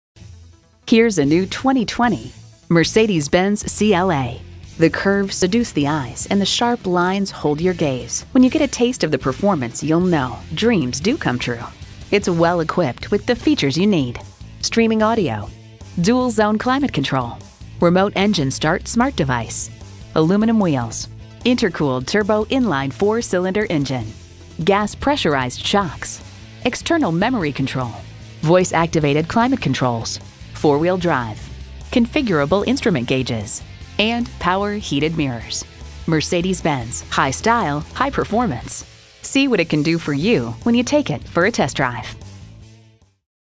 2020 Mercedes-Benz CLA Owings Mills MD Baltimore, MD #800269 - SOLD
Year: 2020
Make: Mercedes-Benz
Model: CLA
Trim: CLA 250
Engine: 2.0L I4 Turbocharged
Transmission: 7-Speed Automatic
Color: Gray
Interior: Black
Stock #: 800269
VIN: WDD5J4HBXLN026280

Address:
9727 Reisterstown Road